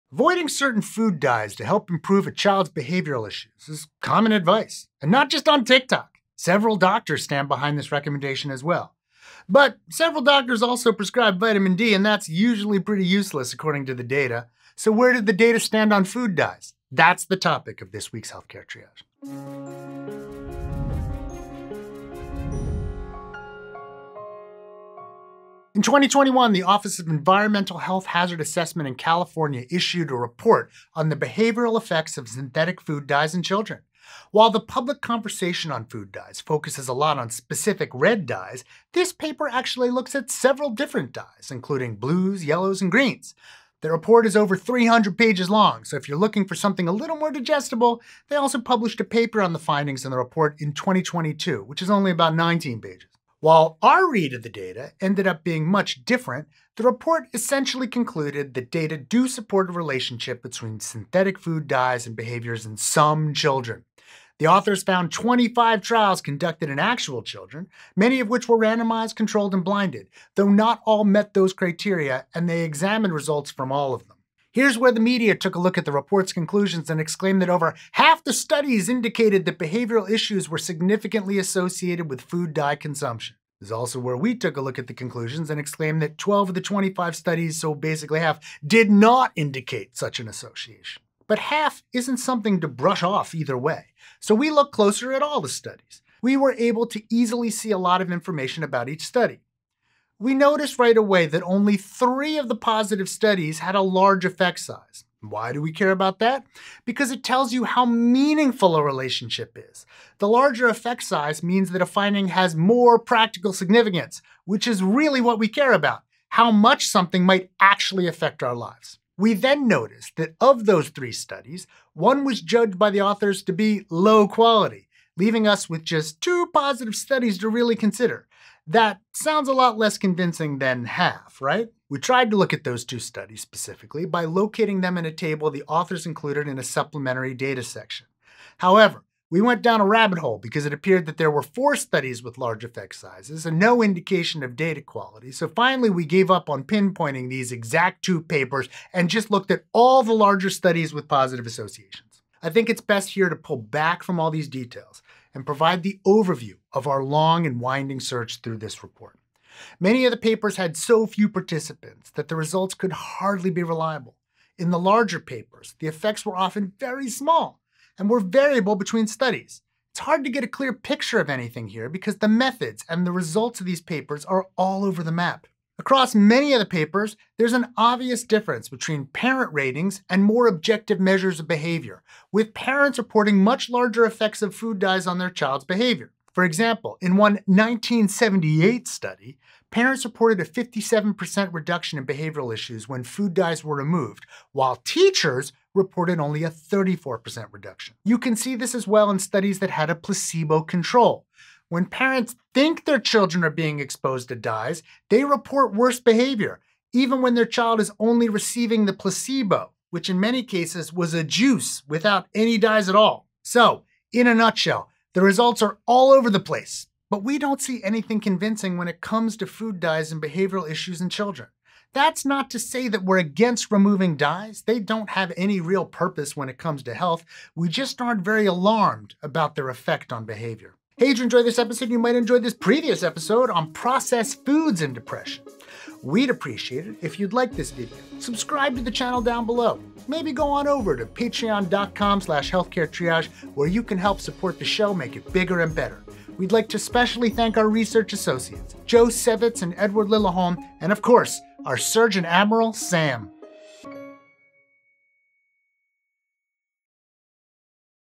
Do Food Dyes Make Kids Hyperactive?
Avoiding certain food dyes to help improve a child’s behavioral issues is common advice, and not just on TikTok! Several doctors stand behind this recommendation as well. But several doctors also prescribe vitamin D, and that’s usually pretty useless according to the data. So where do the data stand on food dyes?

Other Healthcare Triage Links:
2.
3.
4. Aaron's book "The Bad Food Bible: How and Why to Eat Sinfully" is available wherever books are sold, such as Amazon

viafilms/Creatas Video+/Getty Images Plus
SolStock/Creatas Video/Getty Images Plus